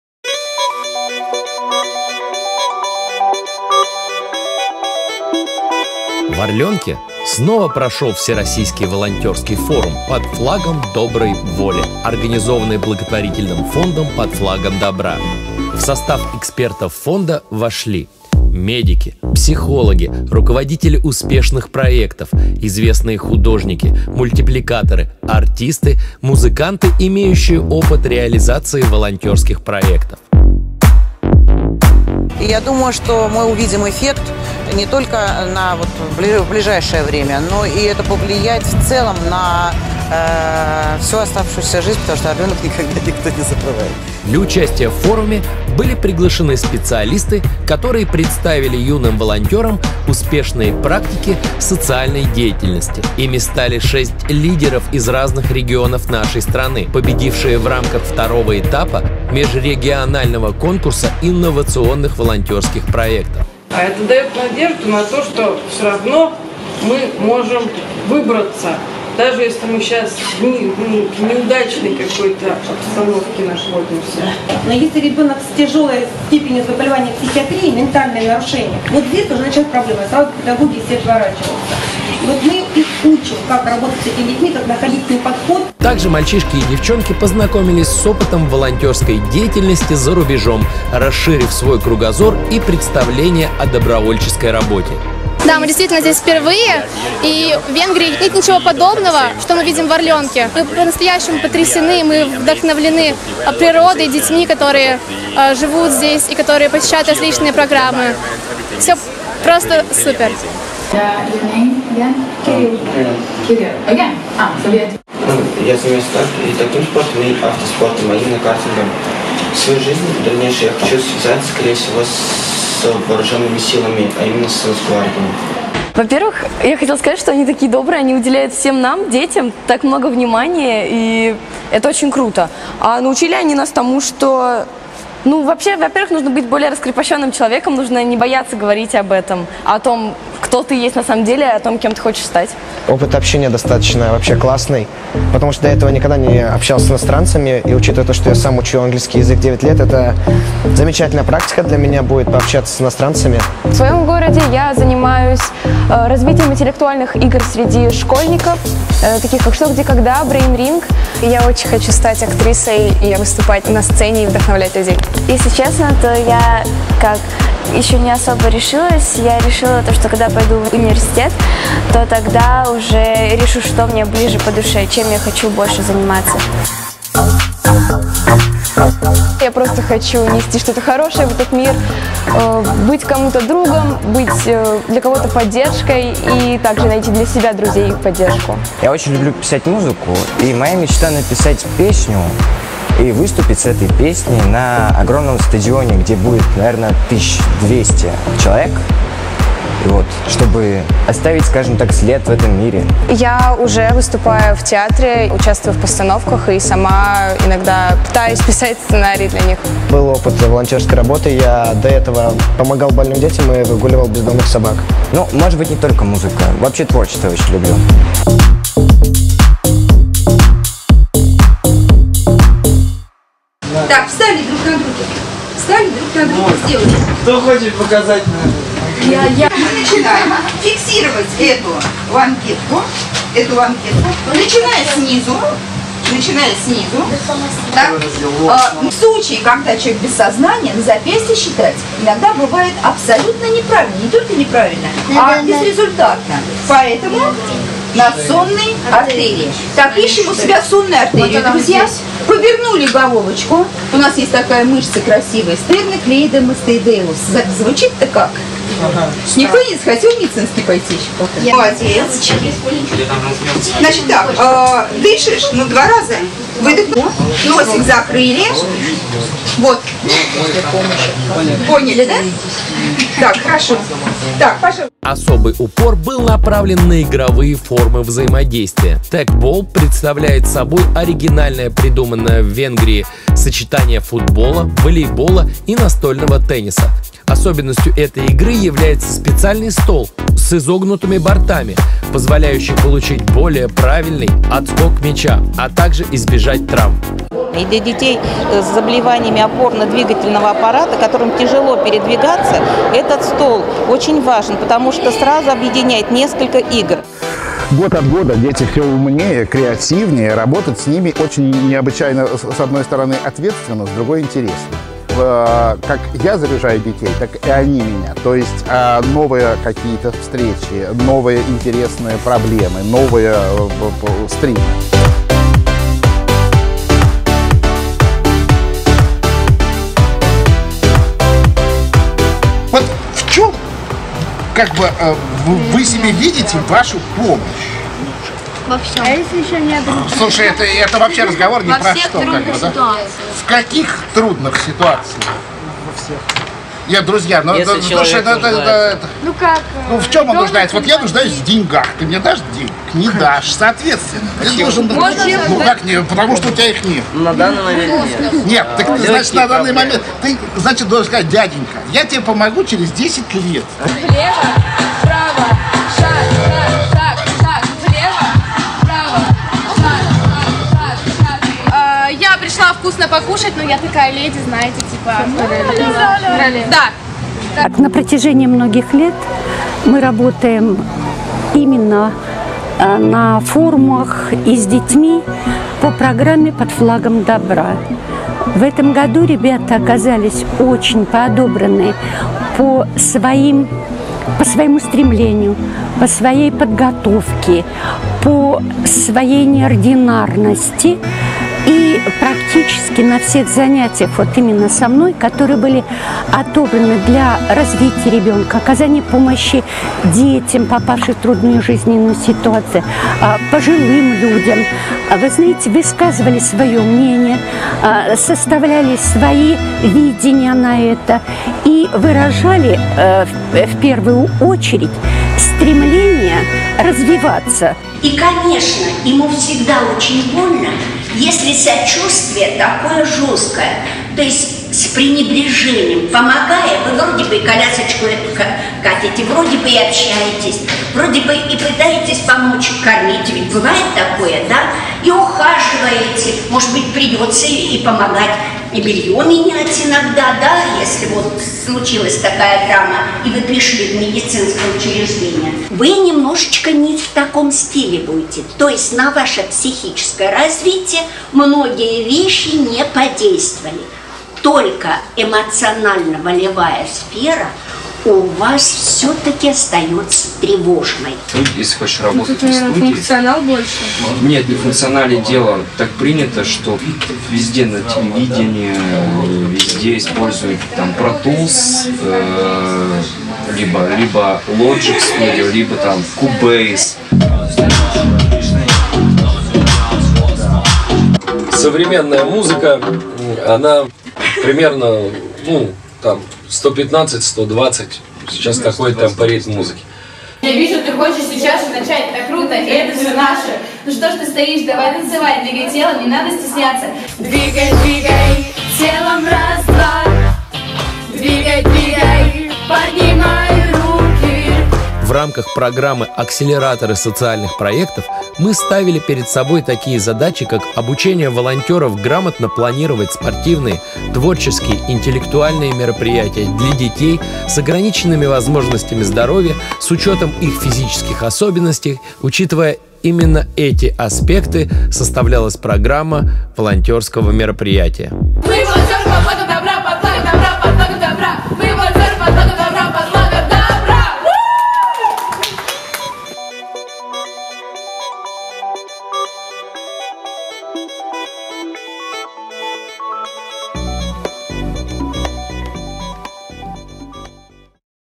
Форум Волонтеров в Орленке 2018
Документальный фильм Михаила Гребенщикова о Волонтерской смене в ВДЦОрленок